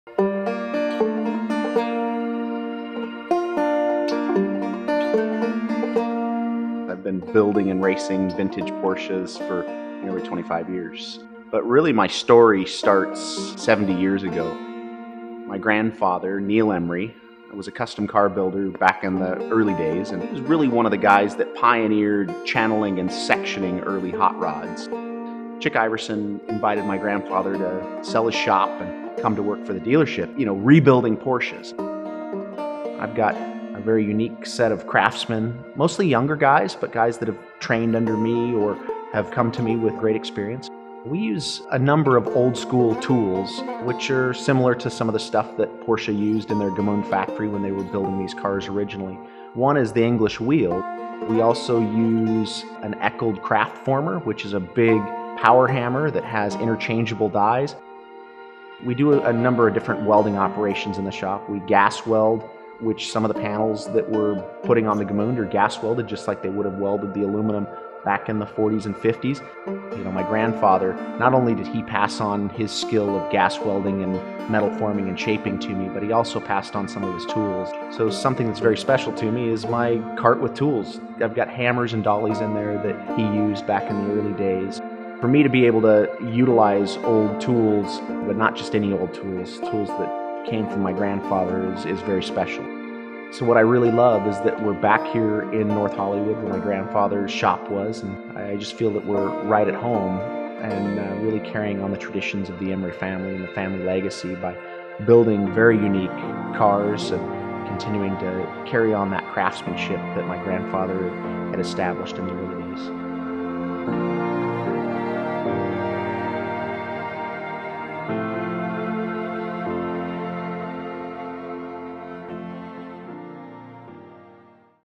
Emory Motorsports Introduction
Thank you to Ideaville for making this greeting card for us. We hope you like it.

Emory Motorsports builds the most iconic Porsche 356s on the planet from factory original cars. Our cars benefit from the best bare-metal rotisserie restoration, body work and paint. We build only two types of cars, Outlaws and Specials.